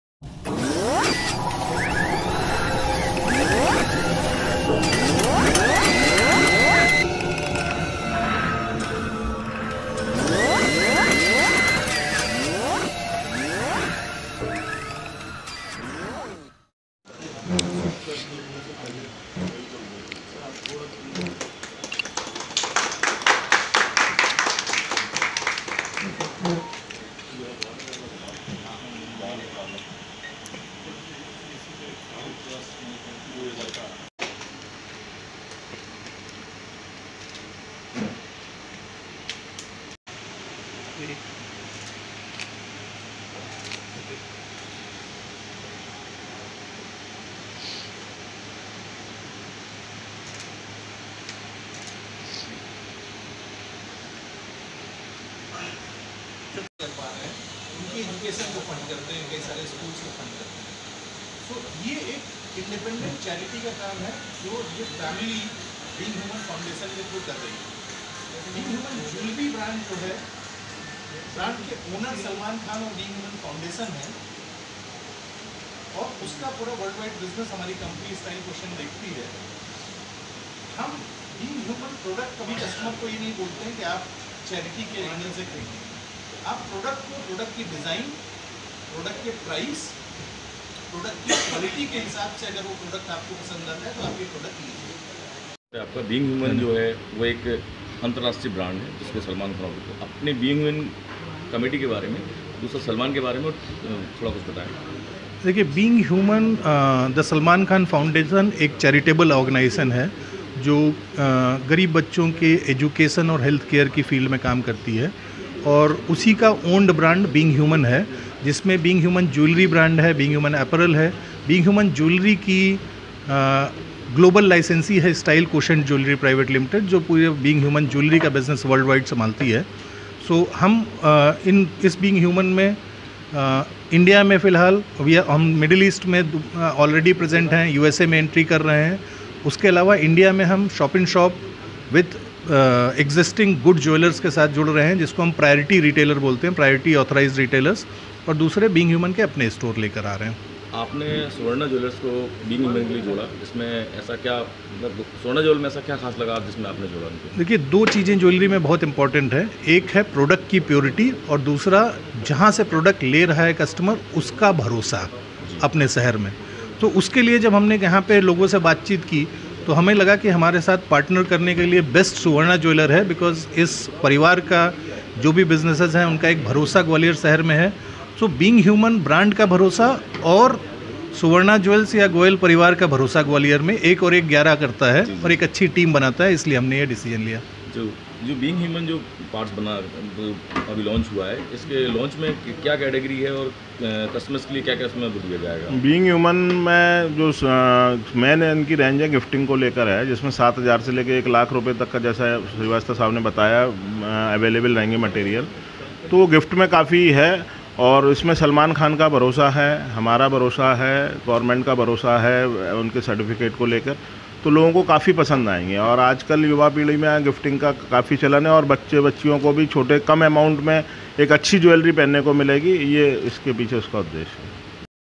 सुवर्णा ज्वेल्स पर लांच हुई सलमान खान की "बीइंग ह्यूमन ज्वेलरी" ग्वालियर 24 अगस्त, ग्वालियर एवं चं
सुवर्णा ज्वेल्स पर लांच हुई सलमान खान की "बीइंग ह्यूमन ज्वेलरी"

ग्वालियर 24 अगस्त,  ग्वालियर एवं चंबल संभाग के एकमात्र 100% बी.आई.एस हॉलमार्क व सर्टिफाइड डायमंड ज्वेलरी शोरूम सुवर्णा ज्वेल्स, सराफा बाजार पर सलमान खान द्वारा संचालित बिंग हुमन ज्वेलरी लॉन्चिंग की गई।

बीइंग ह्यूमन ज्वेलरी के एमडी श्री संतोष श्रीवास्तव ने बताया कि बीइंग ह्यूमन ज्वेलरी, बीइंग ह्यूमन "दी सलमान खान फाउंडेशन" का एक ब्रांड है और बीइंग ह्यूमन फाउंडेशन ज़रूरतमंद बच्चों की शिक्षा एवं स्वास्थ्य संबंधी समस्याओं के लिए आर्थिक मदद करती है। श्री श्रीवास्तव ने यह भी बताया की बीइंग ह्यूमन ज्वेलरी के मालिक सलमान खान का कहना है की आप हमारी ज्वेलरी सही क्वालिटी, अच्छे डिज़ाइन व उचित मूल्य के आधार पर खरीदें न की सामाजिक कार्यों के खर्च के आधार पर।

श्री सलमान खान ने एक संदेश के माध्यम से ग्वालियर और चंबल संभाग के लोगों को सुवर्णा ज्वेल्स, सराफा बाजार, लश्कर, ग्वालियर में आकर बीइंग ह्यूमन ज्वेलरी के डिजाइंस को देखने और परखने के लिए एक आमंत्रण दिया है।

सुवर्णा ज्वेल्स के संचालक श्री अखिलेश गोयल ने बताया कि शोरूम पर बीइंग ह्यूमन ज्वेलरी की लॉन्चिंग म.प्र. चेम्बर ऑफ कॉमर्स अण्ड इंडस्ट्री, ग्वालियर के अध्यक्ष श्री विजय गोयल जी के कर कमलों द्वारा की गई, लॉन्चिंग में बीइंग ह्यूमन ज्वेलरी के एमडी श्री संतोष श्रीवास्तव, वाइस प्रेसिडेंट श्री राजीव अग्रवाल, सुवर्णा ज्वेल्स से श्री अजय गोयल, श्री अखिलेश गोयल, श्री शरद गोयल आदि उपस्थित थे।खबर ग्रामीण का हक नेशनल न्यूज जितेंद्र सिंह कैम विनोद सोनी ग्वालियर मध्य प्रदेश from 😊😊